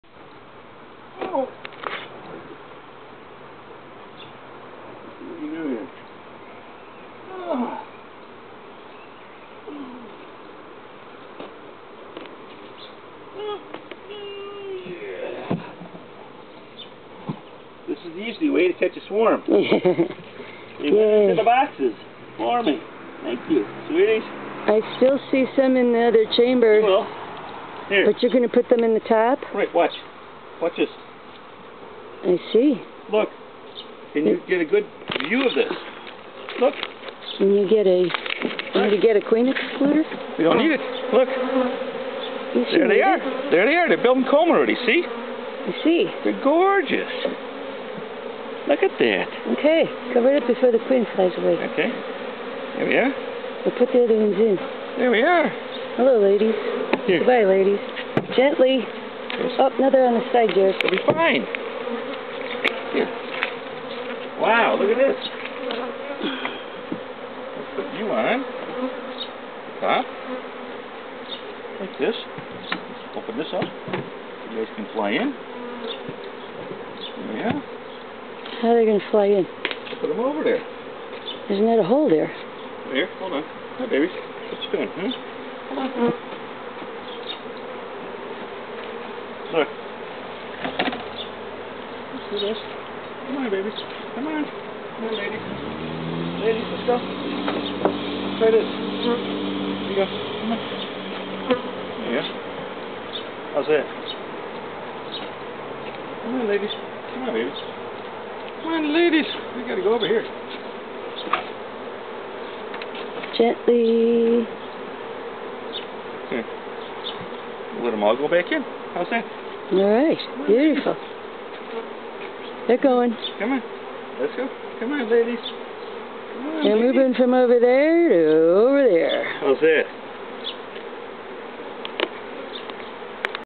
easy honeybee swarm
This new colony is what you call an Easy Swarm!  The swarm settled in some older chambers that we were intending to clean up and recondition.  Well, not this one!!!
No fuss - no muss - the colony just flew right in and made themselves at home. We will move them to a nice fresh hive and frames. lucky, huh?